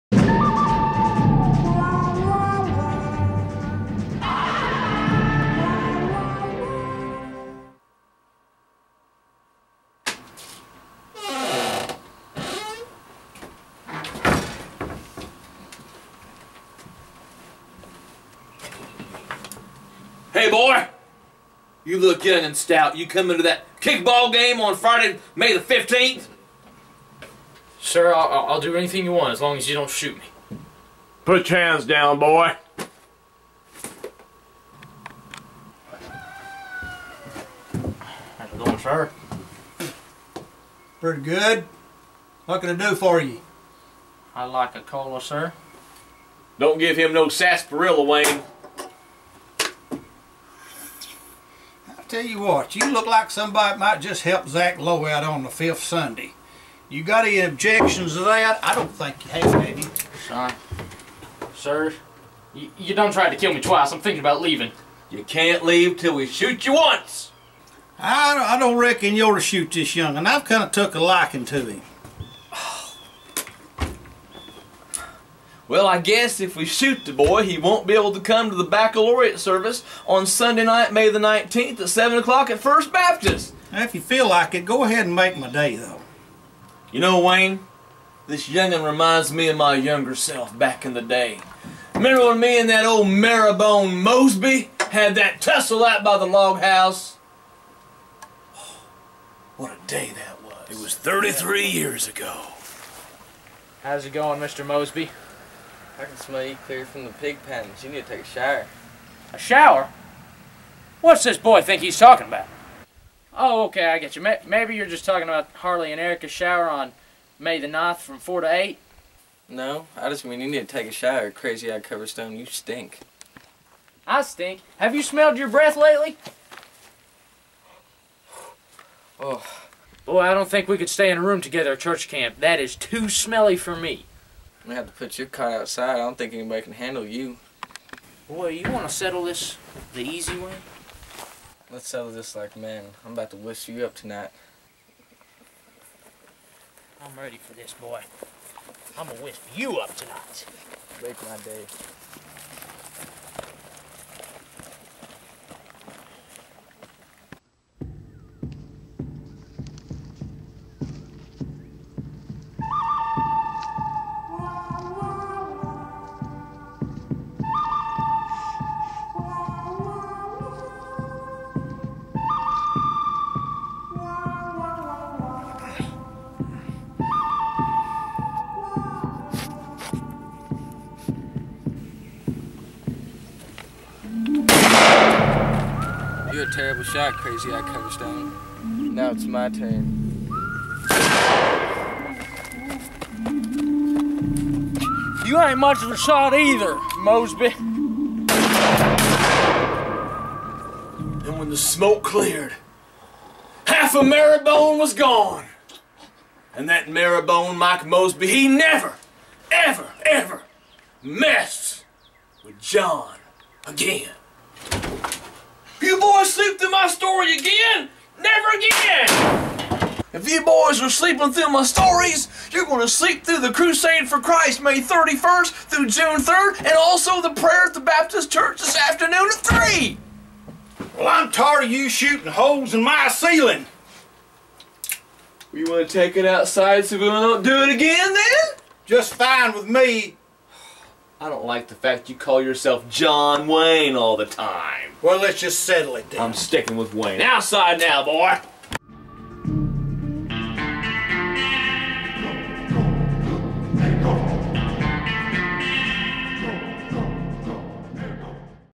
LWMvideo Announcement #16: Wild West
Crazy Eyed-Coverstone recalls the day him and Marrowbone Mosby fought it out in the wild west of Cumberland County.